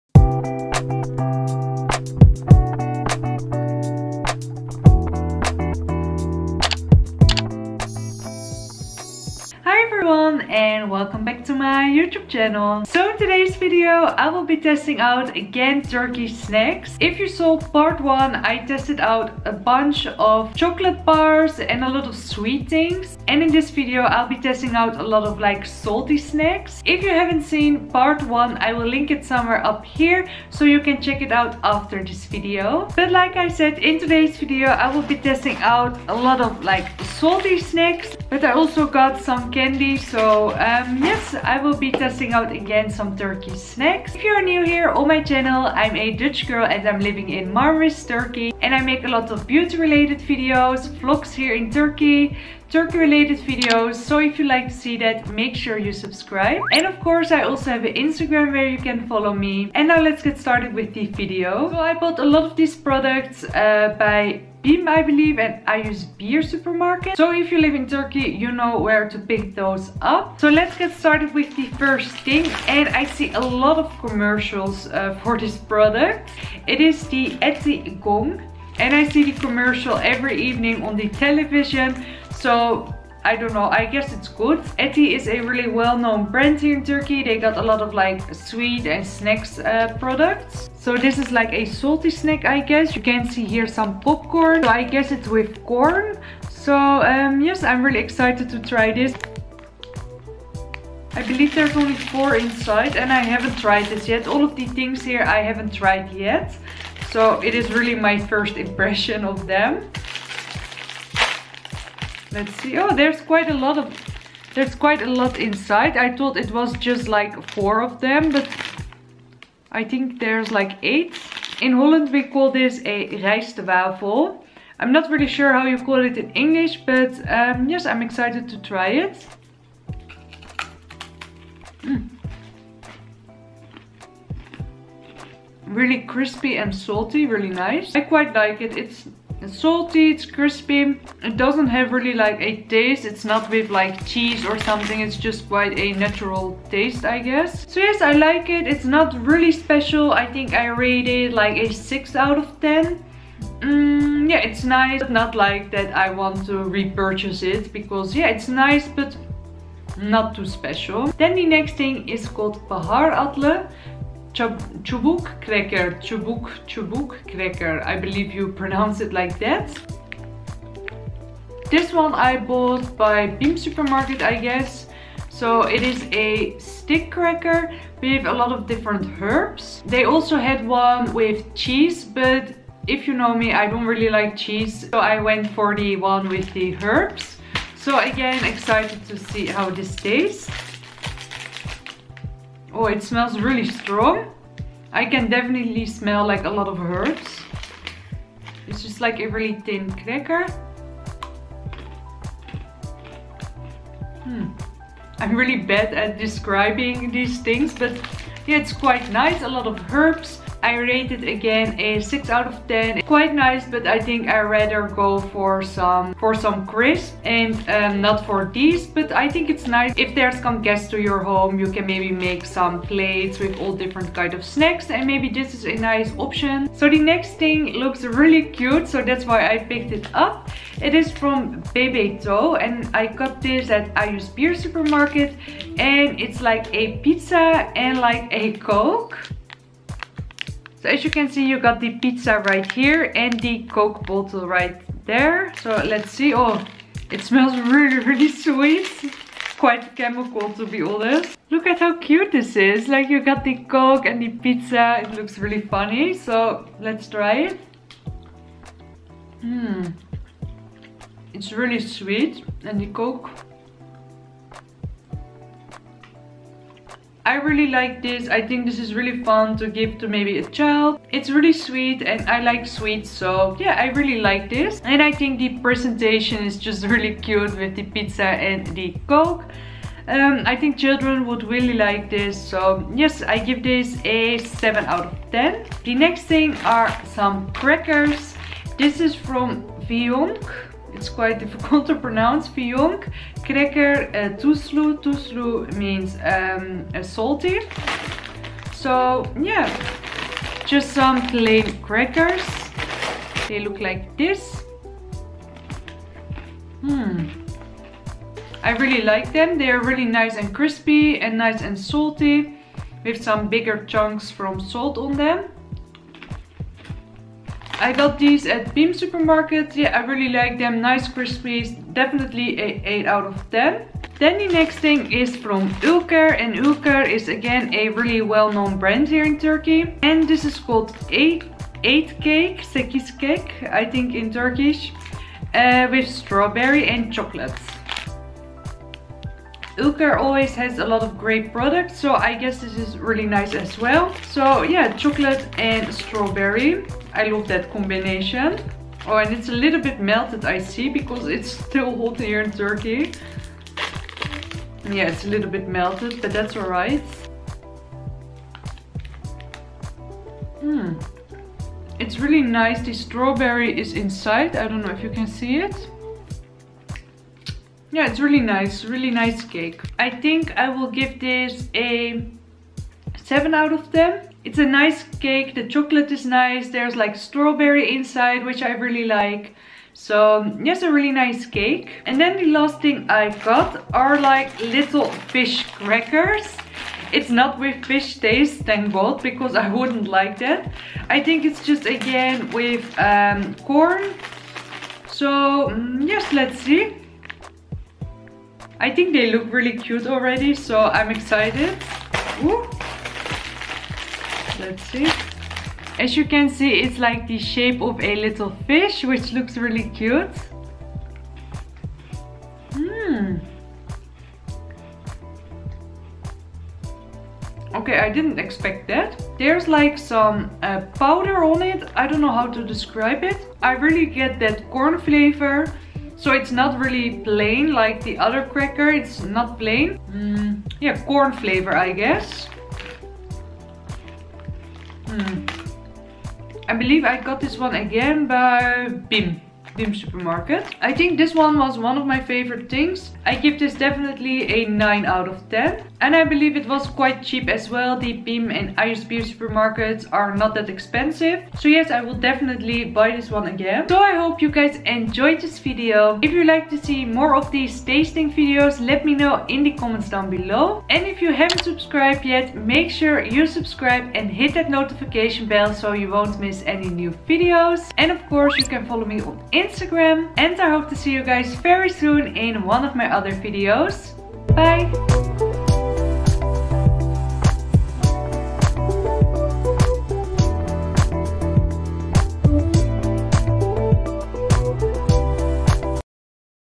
TRYING TURKISH SNACKS 2020🇹🇷PART 2 | My Pretty Everything
DUTCH GIRL TRIES TURKISH SNACKS🇹🇷PART 2 | My Pretty Everything
Hi loves!💕
In today's video I'll be testing out a lot of Turkish snacks!🇹🇷I hope you enjoy it!